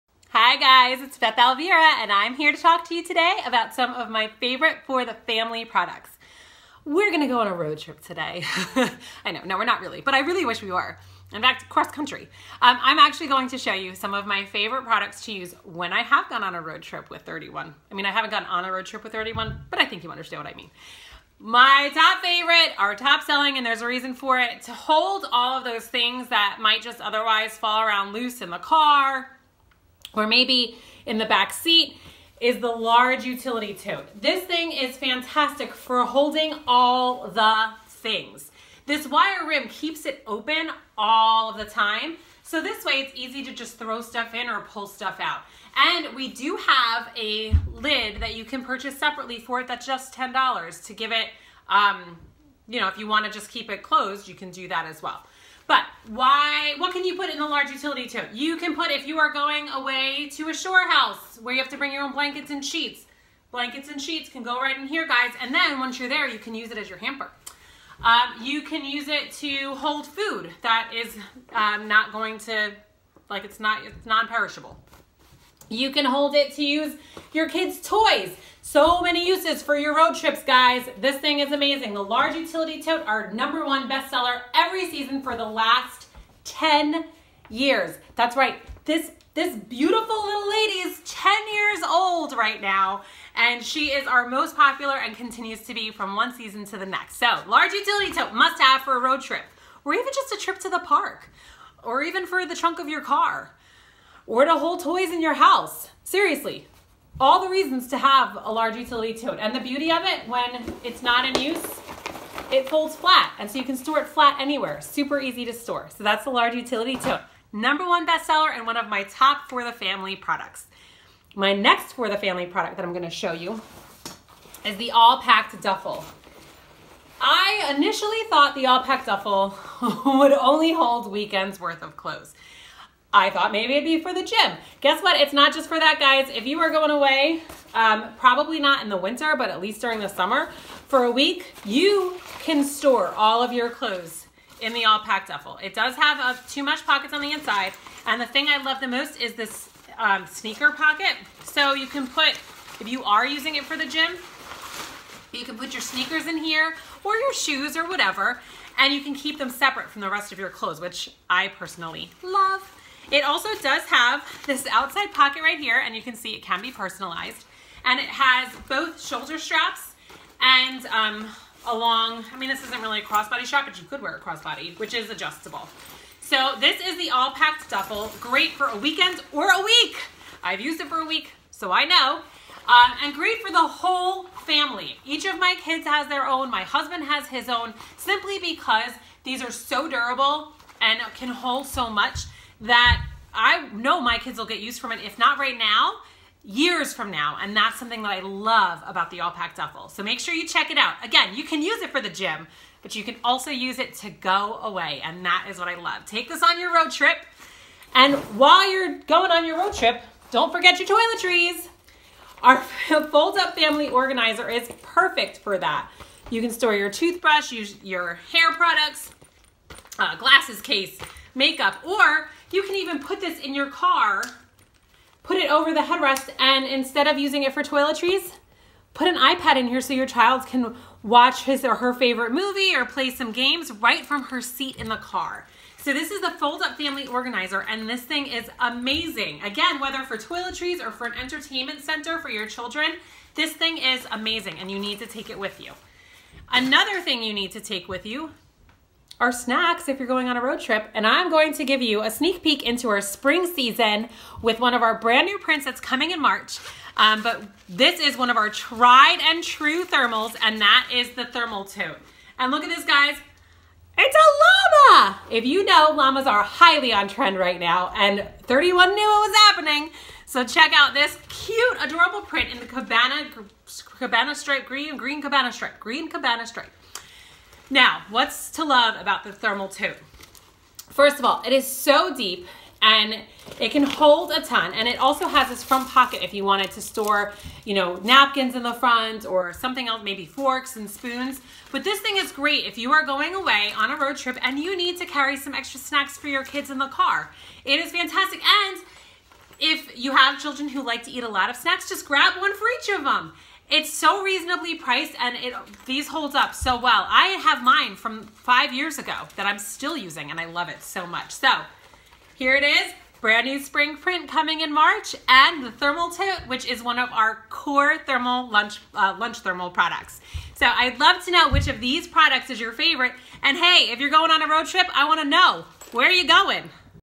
For the Family favorites! (02/19)
Some of my favorite Road-Trip-Ready products that the whole FAMILY will love! To see current prints, patterns, and personalization, head on over to your hostess’s party link or to bethsbagsandbeyond.com.

Products highlighted in this video:
- the Large Utility Tote
- All Packed Duffle
- Fold-Up Family Organizer
- Thermal Tote